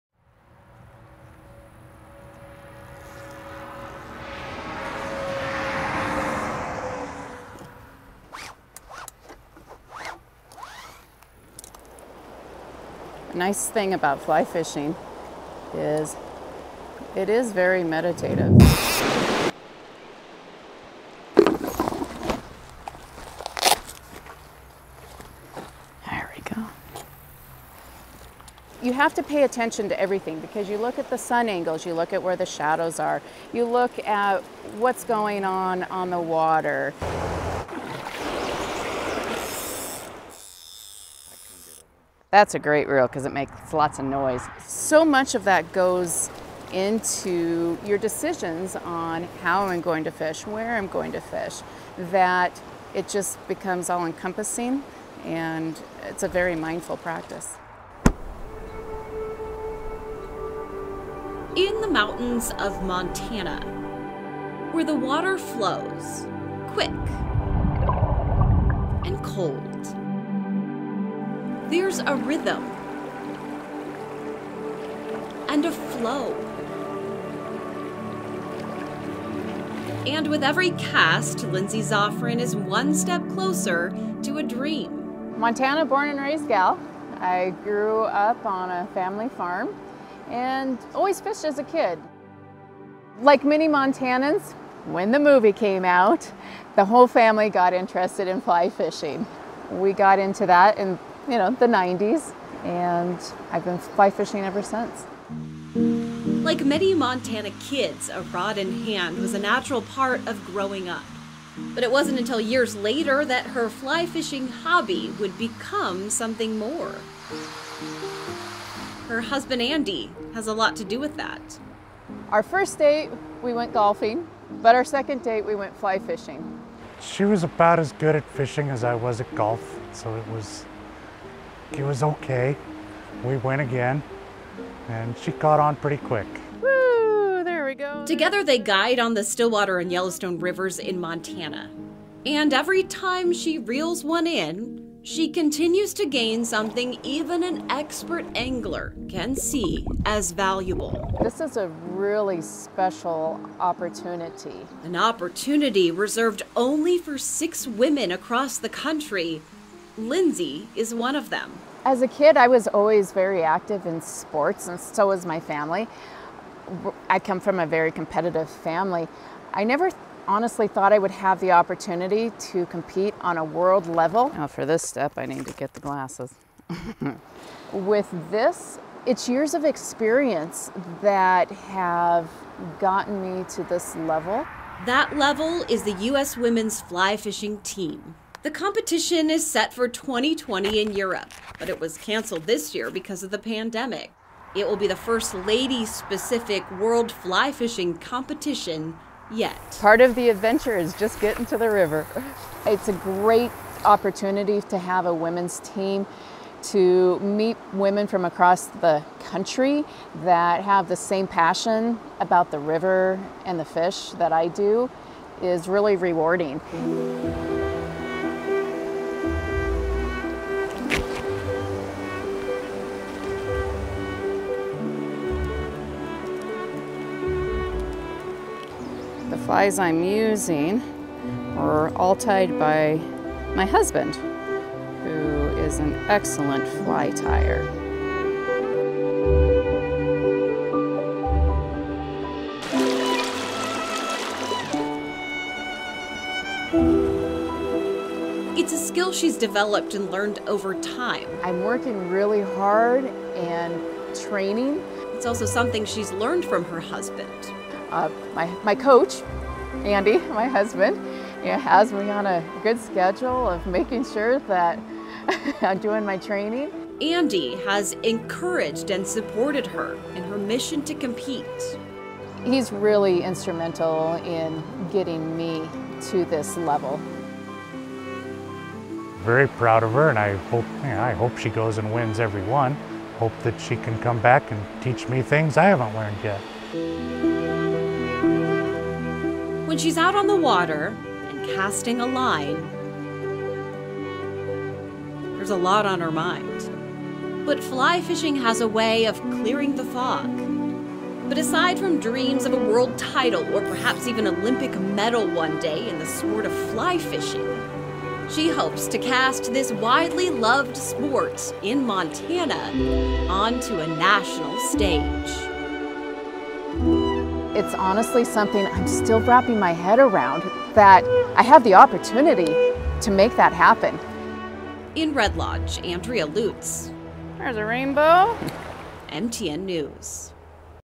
Red Lodge woman lands a spot on US Ladies Fly Fishing team
The opportunity to compete on the world level is reserved for only six women across the country and Lindsay Szofran is one of them.